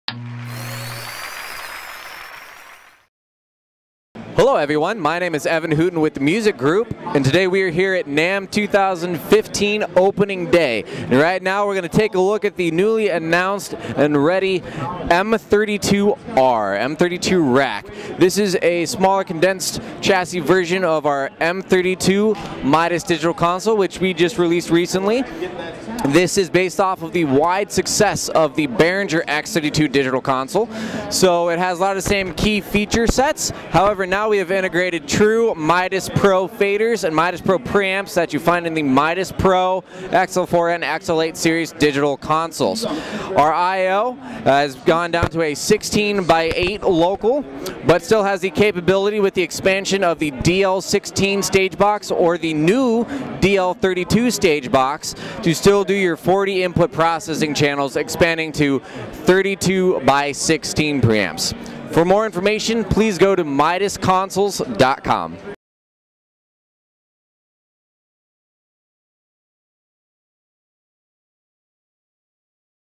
Midas M32R Mixer
The new Midas M32R puts the power of the larger M32 console. into a rack mount desktop design, with 40 input channels (16 Midas mic preamps), 17 motorized faders, 25 mix buses, AES50 networking for up to 96 inputs/96 outputs in an open architecture design for future 96 kHz operation.